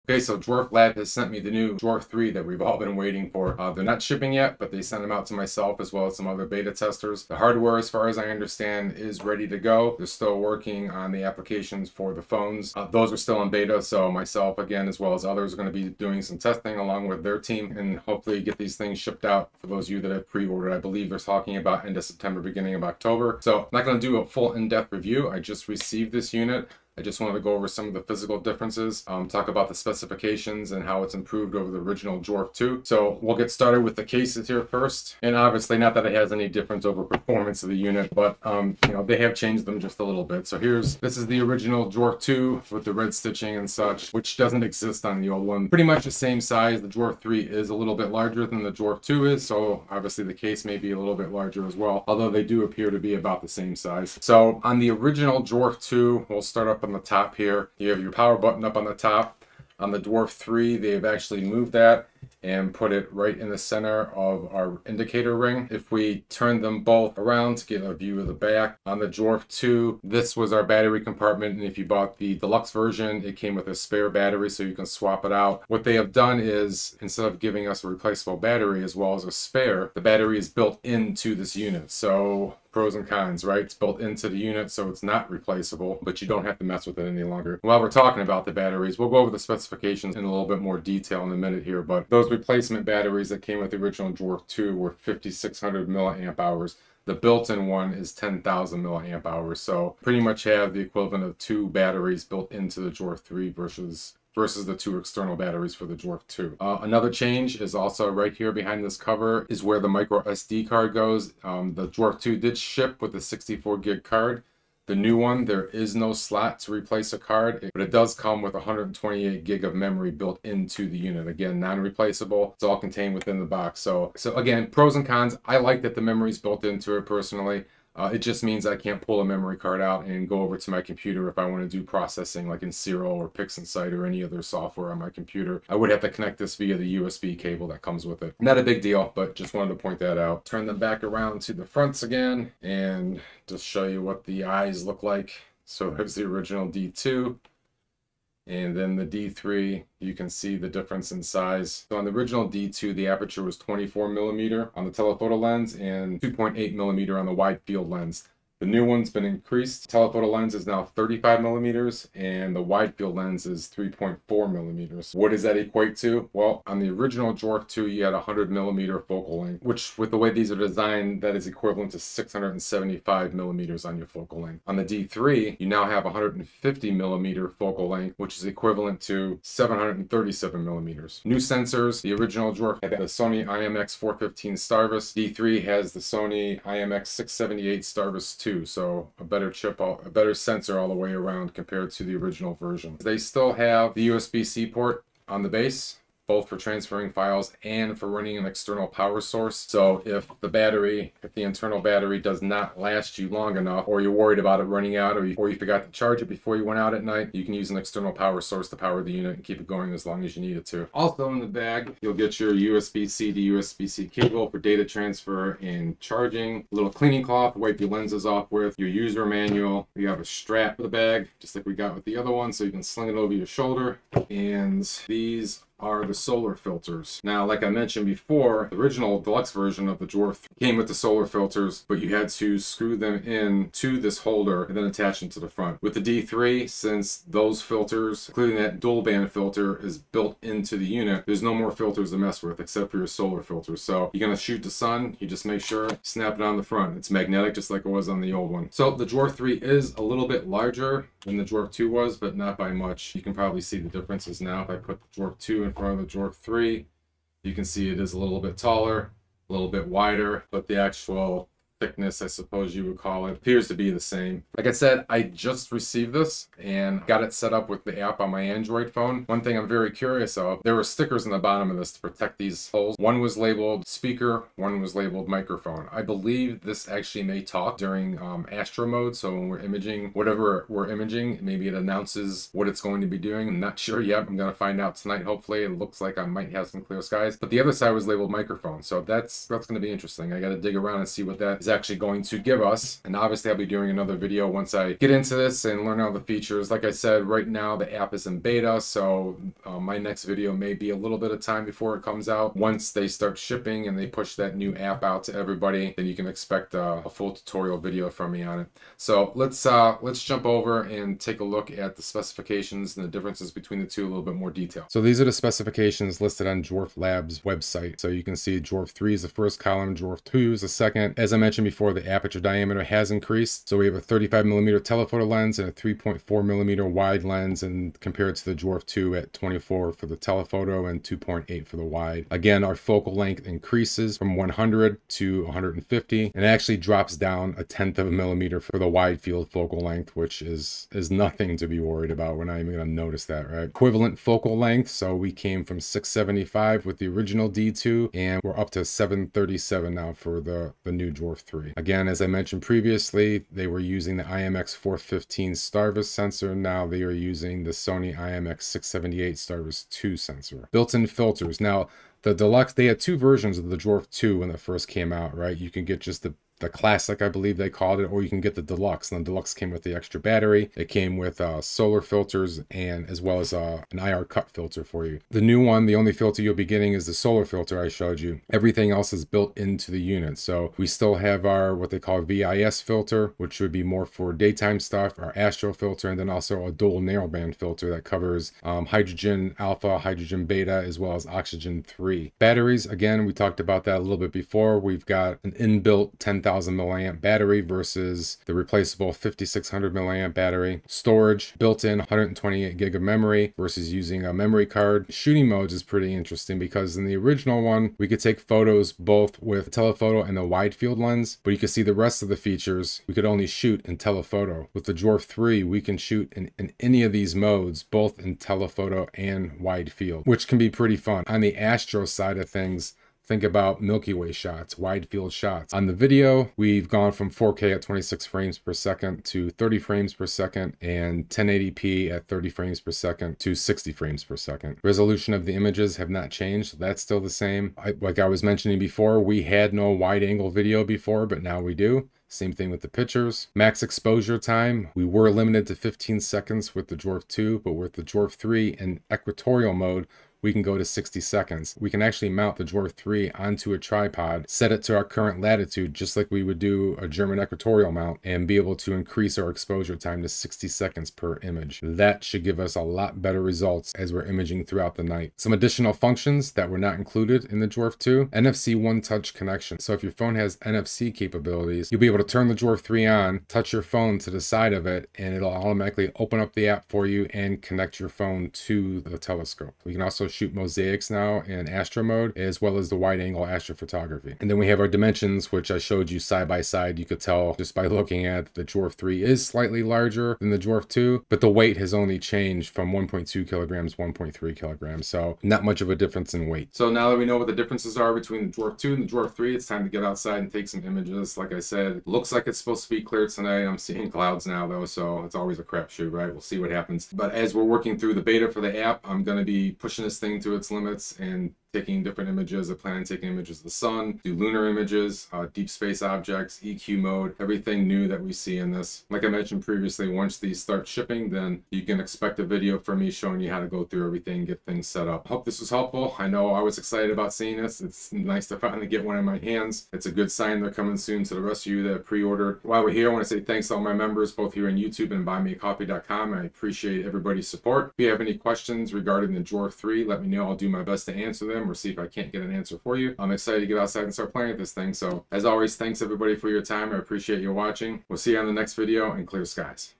Comparing the Dwarf 3 and Dwarf 2 Smart Telescopes
In this video I show you the differences and improvements between the original Dwarf 2 and the new Dwarf 3 Smart Telescopes.